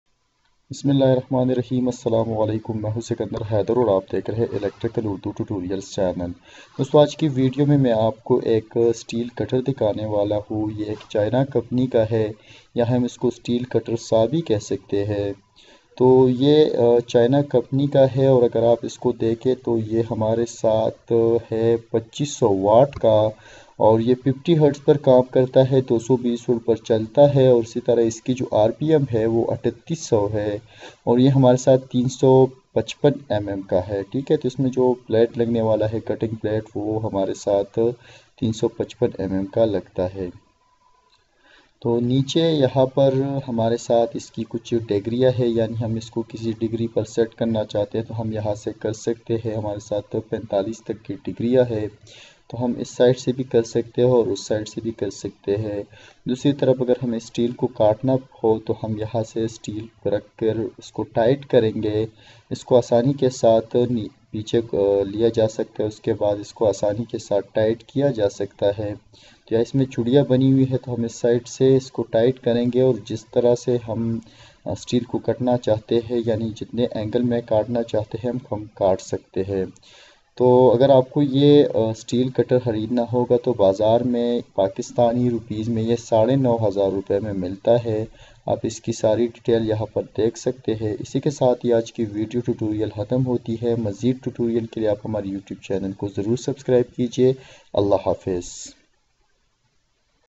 Metal Cutting Circular Saw Review | Price of Steel China Steel Cutter
This video is about the steel cutter price and review of chine metal cutting circular saw.